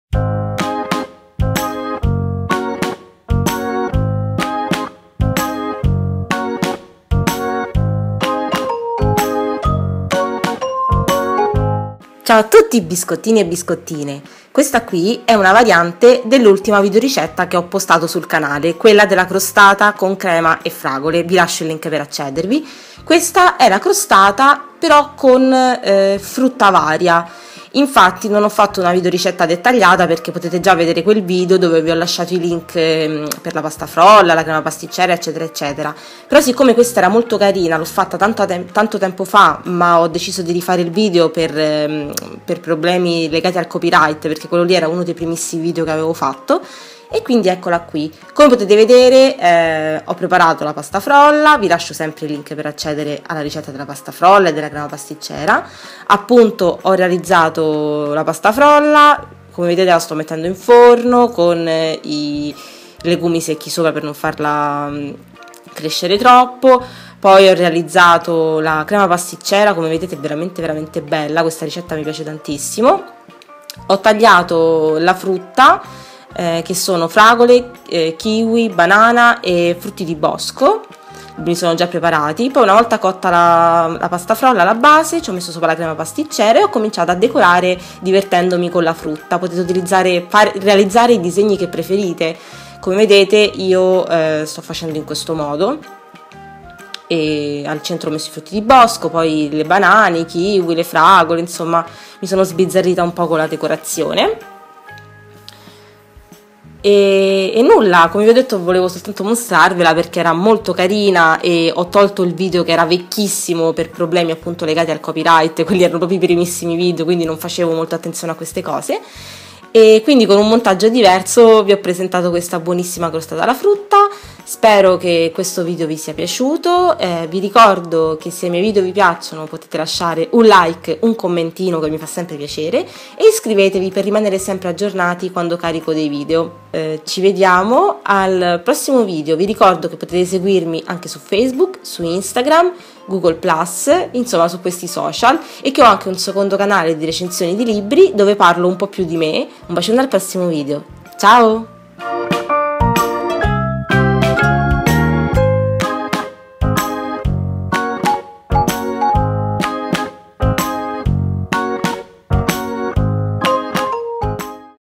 CROSTATA DI FRUTTA [Le dolcezze di Viky]
Una coloratissima CROSTATA DI FRUTTA!
METTI IL VIDEO IN HD PER VISUALIZZARLO AL MEGLIO!

Ingredienti:
pasta frolla
crema pasticcera
frutta a piacere
gelatina per torte